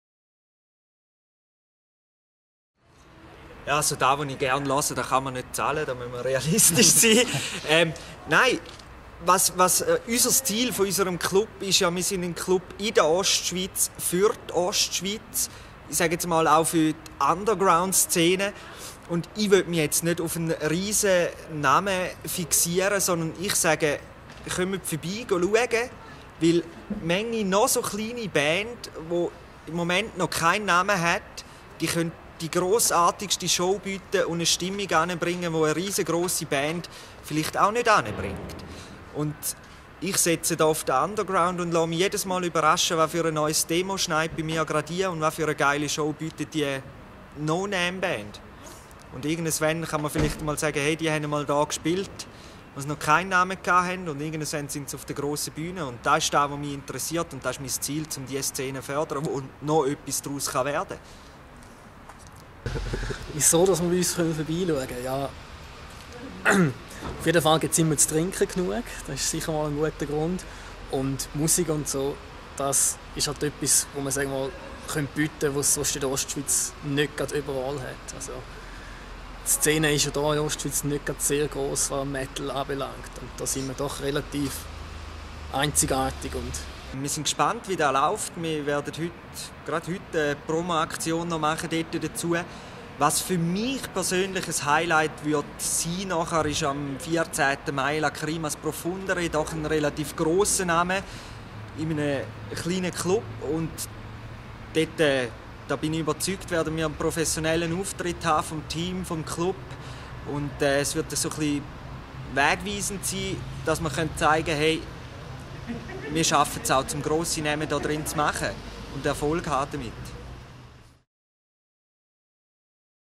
metalvetia_herisau-1.mp4
Interview mit Flo, Raouf und Nico vom Metalvetia Metalclub in Herisau.